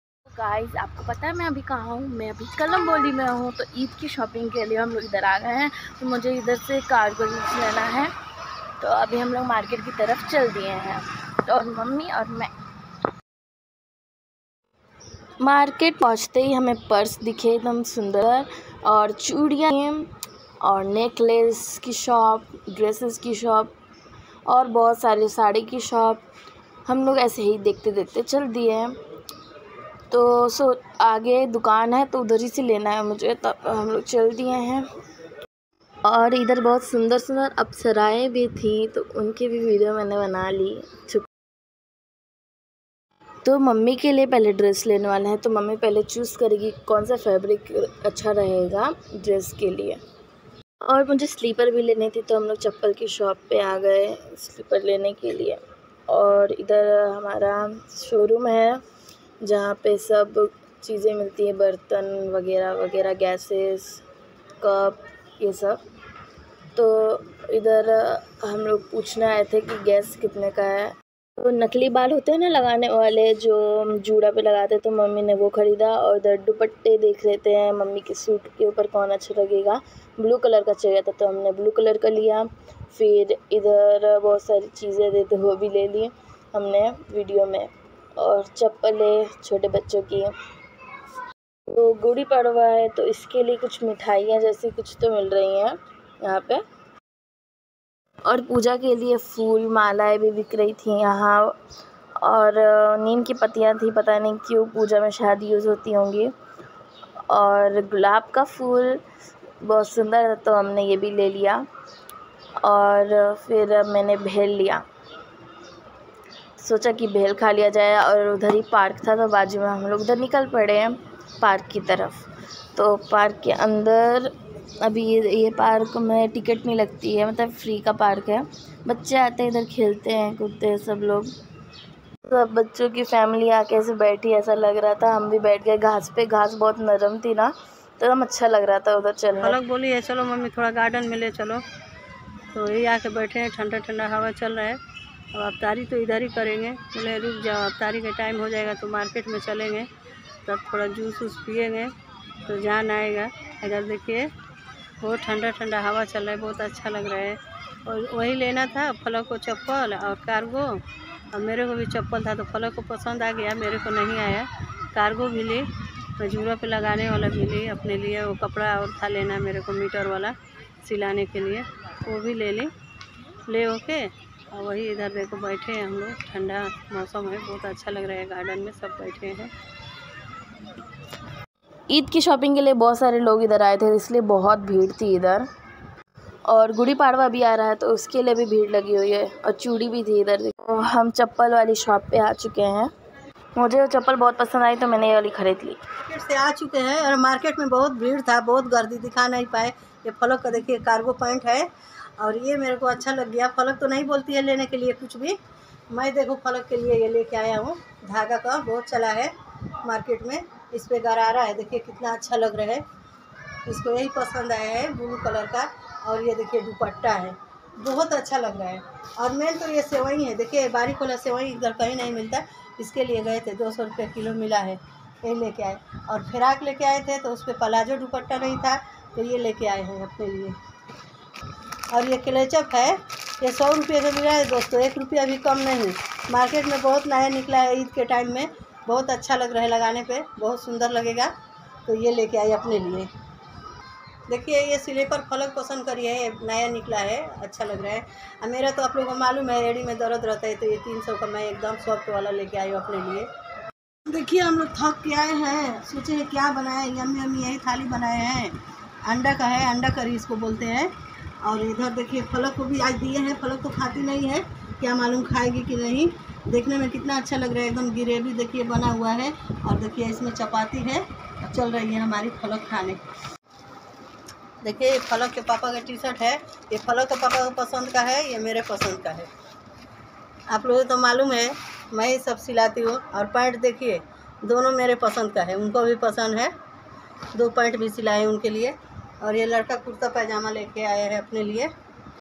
Eid or gudi padwa ke festival pe sab shopping kar rahe hai
guys to ham log Eid ki shopping ke liye yahan aaye Hain are bhai kahan aaye Hain kalamboli aaye hain
abhi ham Eid ki shopping ke liye aaye hain aur gudi padwa bhi a chuka hai to baki sab log bhi iski bhi shopping kar rahe hai, bahut bhid hai idhar
idhar sari chijen sasti milti hai

share please
comment me

by guys 👋💜